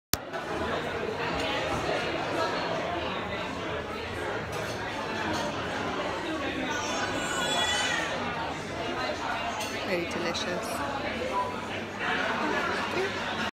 The Ivy Brighton | Rachel Evans Barbie
Mid-Week outfit shopping I pop into The Ivy restaurant Brighton for Lunch of Cod and Chips,

Agent Susan Scott Lookalikes
International Bookings Scarlett Entertainment
IMDb Rachel Evans